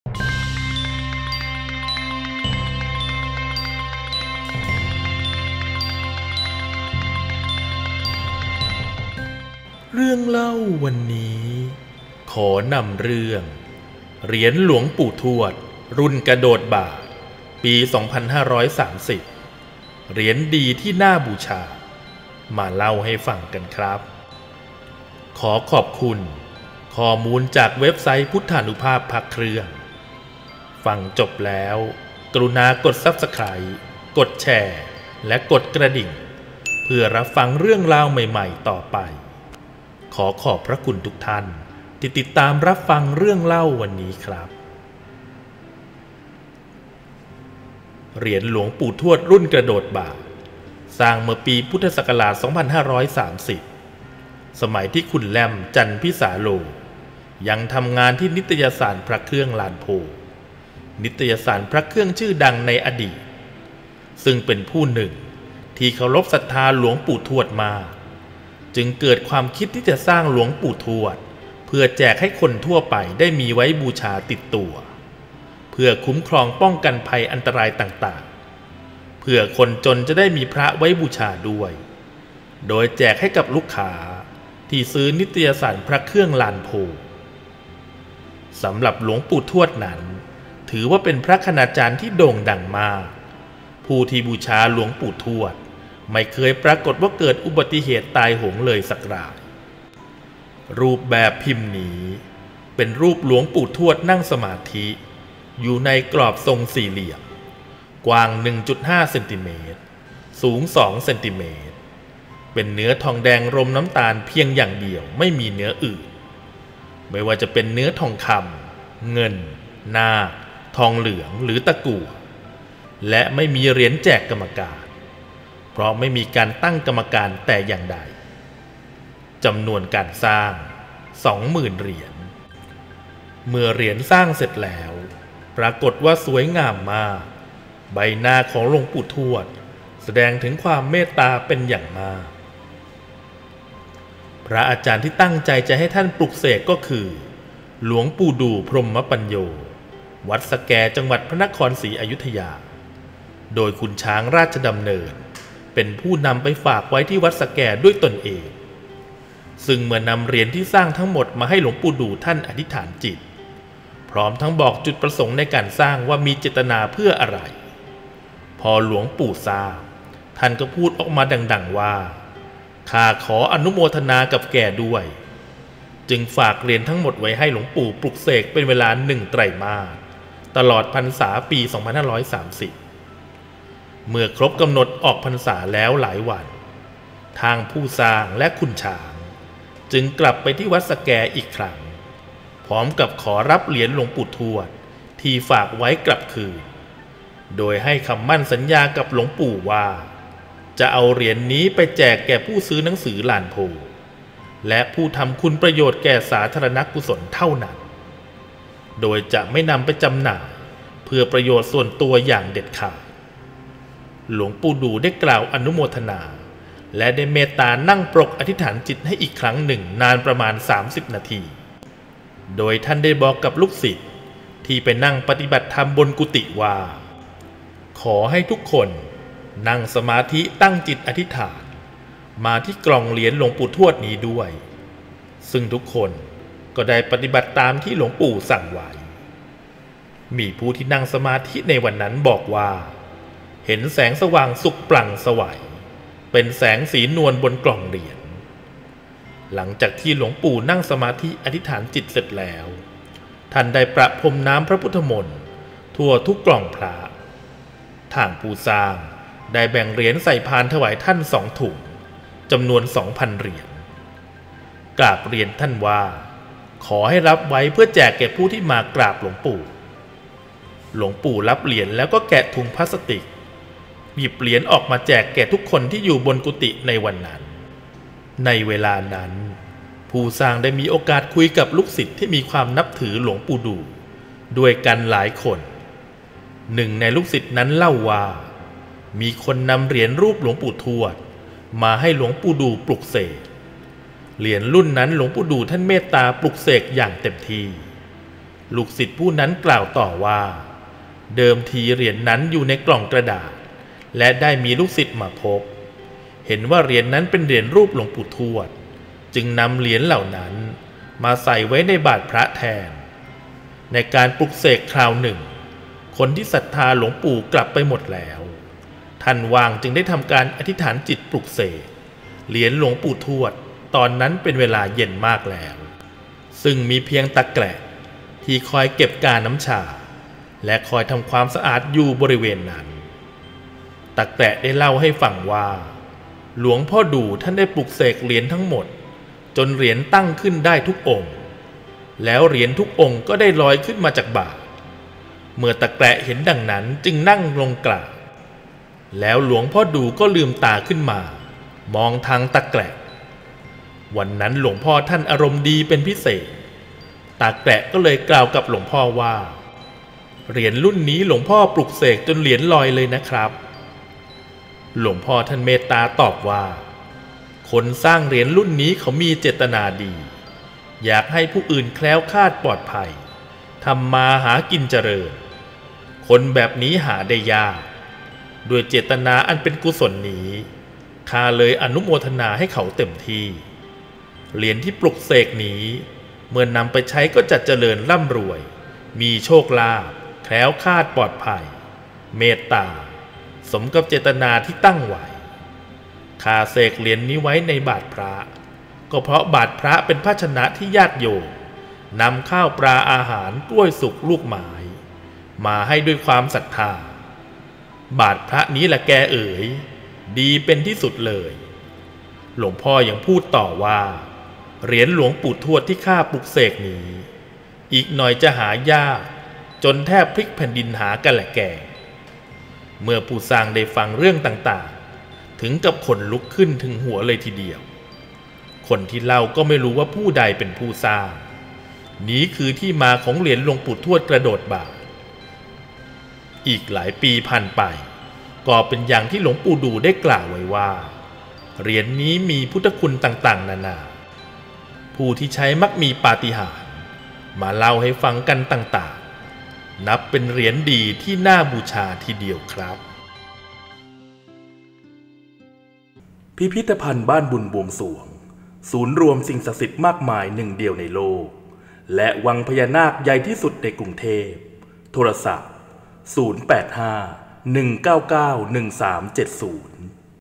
เหรียญหลวงปู่ทวด รุ่นกระโดดบาตร 2530 เหรียญดีที่น่าบูชา | เรื่องเล่าวันนี้
เรื่องเล่าวันนี้ขอนำเสนอเรื่องราวของ เหรียญหลวงปู่ทวด รุ่นกระโดดบาตร 2530 เหรียญดีที่น่าบูชา มาเล่าให้ฟังกันครับ